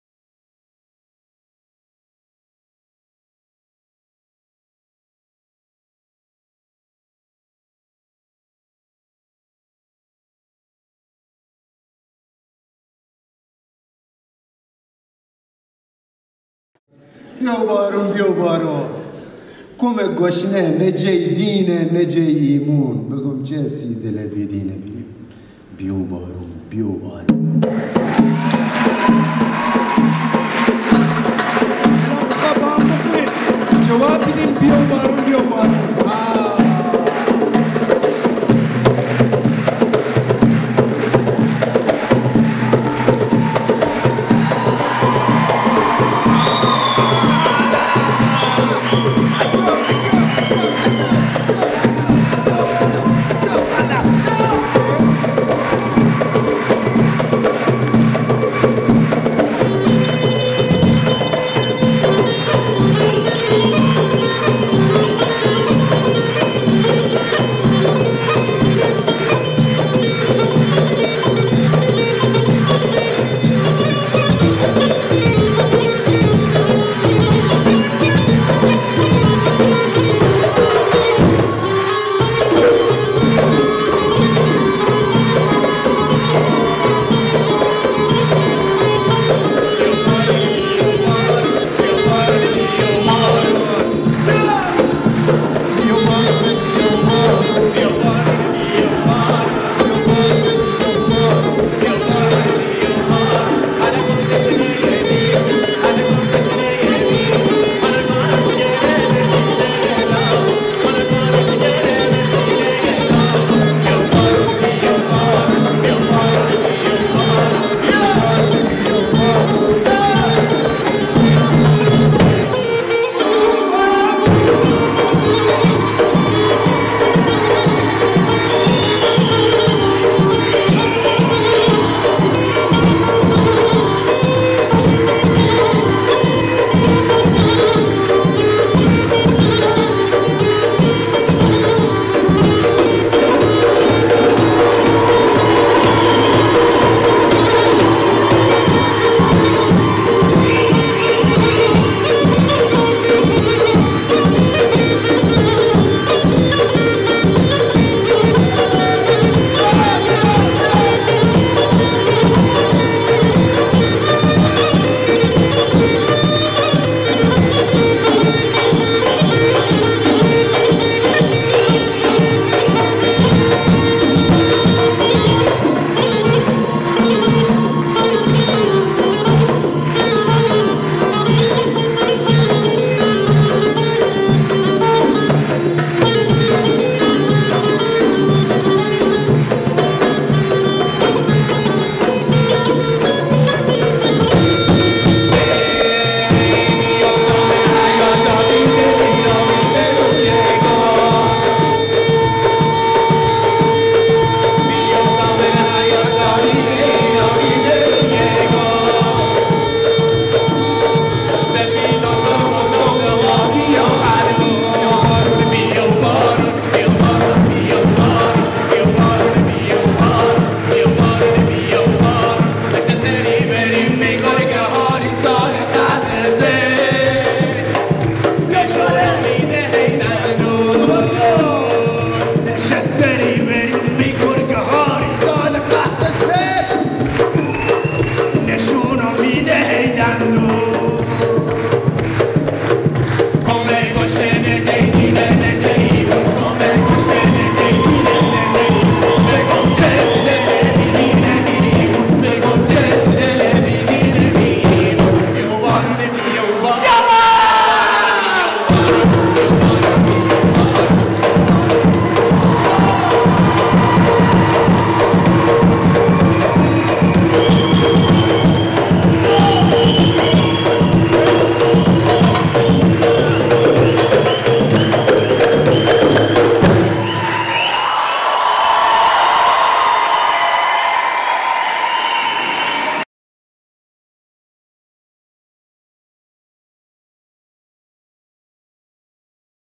Festival of Arts' فستیوال هنر The Eternal Persian Gulf Concert Cadogan Hall London 2012
The Eternal Persian Gulf Concert - Shanbehzadeh Ensemble
Saeid Shanbehzadeh / Naghib Shanbehzadeh / Habib Meftah Boushehri
کنسرت خلیج همیشه فارس
سعید شنبه زاده - نقیب شنبه زاده - حبیب مفتاح بوشهری